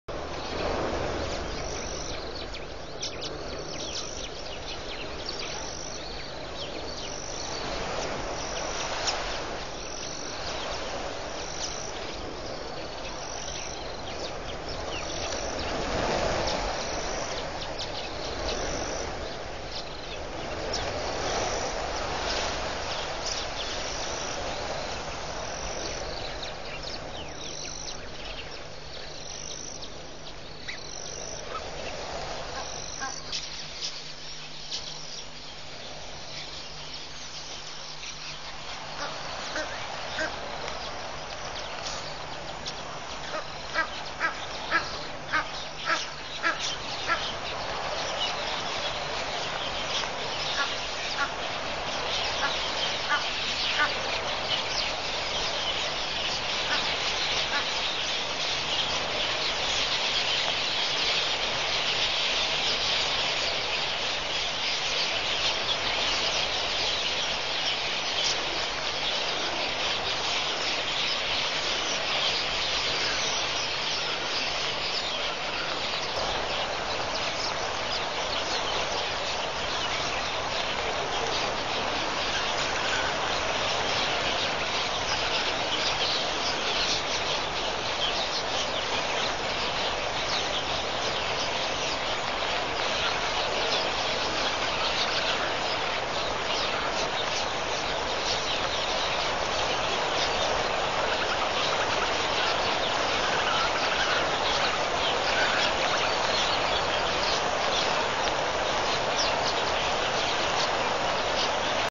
Swarms of Quelea at Pete's Pond October 31, 2015
Hundreds, if not thousands, of red-billed quelea swarm above the water at Pete's Pond, Mashatu Game Reserve, Botswana, Africa. Seen on WildEarth.tv/Ustream.  Edited down from a longer sighting. October 31, 2015